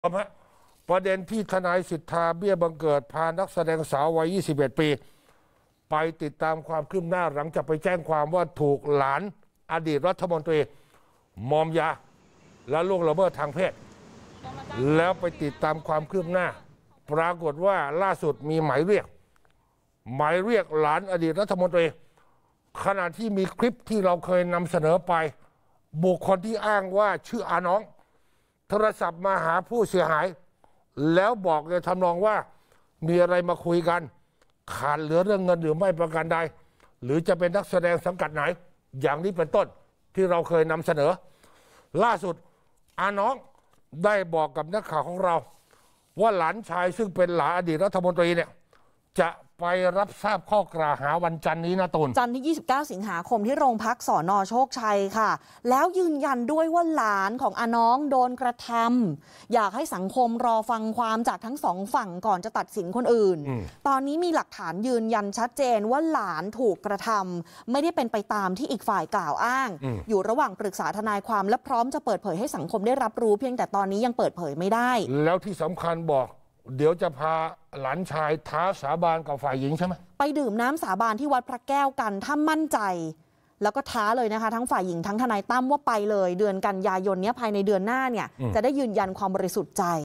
ญาติท้าดาราสาวสาบานวัดพระแก้ว ยันหลานถูกกระทำ วอนฟังความ 2 ฝั่ง พร้อมพบ ตร.จันทร์นี้
‘หลานชายอดีตรัฐมนตรี’ พร้อมพบตำรวจตามหมายเรียกจันทร์นี้ สู้คดีมอมยา-ข่มขืน ดาราสาว  ด้าน ‘อาน้อง’ ของฝ่ายชาย ท้าฝ่ายหญิงดื่มน้ำสาบานวัดพระแก้ว พิสูจน์ความจริง

กรณีที่ทนายตั้ม ษิทรา เบี้ยบังเกิด เลขาธิการมูลนิธิทีมงานทนายประชาชนฯ พาดาราสาว อายุ 21 ปี ไปติดตามความคืบหน้าคดี ถูกนักธุรกิจเจ้าของบริษัทบันเทิง หลานชายอดีตรัฐมนตรี วางยาล่วงละเมิดทางเพศในรีสอร์ทแห่งหนึ่ง ย่านนาคนิวาส 2 (เหตุการณ์วางยา เกิดเมื่อวันที่ 9 สิงหาคม) ซึ่งฝ่ายหญิงแจ้งความที่ สน. โชคชัย เมื่อวันที่ 11 สิงหาคม แต่เห็นว่าคดีไม่คืบหน้า

ต่อมาปรากฏคลิประหว่างที่ผู้เสียหายมาพบทนายตั้ม ญาติผู้ถูกกล่าวหา เรียกตัวเองว่า ‘อาน้อง’ ได้โทรมาหานักแสดงสาว บอกว่ารู้เรื่องที่เกิดขึ้นแล้วรู้สึกไม่สบายใจ กำลังจะให้หลานชายเล่นการเมือง ถ้ามีเรื่องอะไรไม่ดีก็จะเสียหายทั้งคู่ พอจะคุยกันได้มั้ย ถ้ามีปัญหาเรื่องการเงินก็จะช่วย พร้อมยื่นข้อเสนอเรื่องงานในวงการบันเทิง แลกกับการที่จะไม่ดำเนินคดี

ล่าสุด ‘อาน้อง’ แจ้งกับทีมข่าวว่า หลังจากมีข่าวและคลิปการสนทนาของตนกับฝ่ายหญิง ซึ่งเป็นข้อมูลฝ่ายเดียว จากฝ่ายหญิง ตนเครียดจนไม่สบาย จึงไม่พร้อมที่จะให้สัมภาษณ์ชี้แจงรายละเอียด โดยล่าสุดครอบครัวได้มอบหมายเรื่องดังกล่าวให้ทนายความ ซึ่งในวันที่ 29 สิงหาคม อาน้อง และหลานชายซึ่งตกเป็นผู้ถูกกล่าวหา พร้อมทนายความ จะเดินทางไปที่ สน.โชคชัย ตามหมายเรียกตำรวจ เพื่อนำเอาหลักฐานที่มีไปชี้แจง เพื่อแสดงความบริสุทธิ์ใจในมุมของฝ่ายชาย

‘อาน้อง’ ระบุว่า นอกจากจะไปชี้แจงข้อเท็จจริงในทางกฏหมายแล้ว อาน้องในฐานะที่เป็นพุทธศาสนิกชน เตรียมท้าฝ่ายหญิงไปพิสูจน์ความจริง ด้วยการสาบานที่วัดพระแก้ว

“เราไม่อยู่ในเหตุการณ์ เราไม่รู้ความจริง ที่เราฟังจากหลานเรา เขาก็บอกว่าเป็นเรื่องของต่างคนต่างไม่มีสติ และไปด้วยกันด้วยความสมัครใจ อาน้องจึงอยากพิสูจน์ทั้งทางคดี และ อยากจะขอท้าน้องฝ่ายหญิงดื่มน้ำสาบาน ไม่รู้เขาจะกล้าหรือเปล่า ถ้าหลานเราผิดก็ต้องรับผิด ถ้าฝ่ายหญิงผิดก็ว่าไปตามผิด เราจะไม่ใส่ความใคร ที่ผ่านมาหลานชายเป็นคนดีมาก ไม่เคยมีประวัติด่างพร้อย” อาน้องกล่าว

ส่วนเรื่องหลานชายจะมาเล่นการเมืองอาชีพ ระดับประเทศ เป็นเรื่องอนาคตอีกไกล เพราะปัจจุบันเขายังอายุยังน้อย แต่ครอบครัวพร้อมสนับสนุนให้ทำประโยชน์กับบ้านเมือง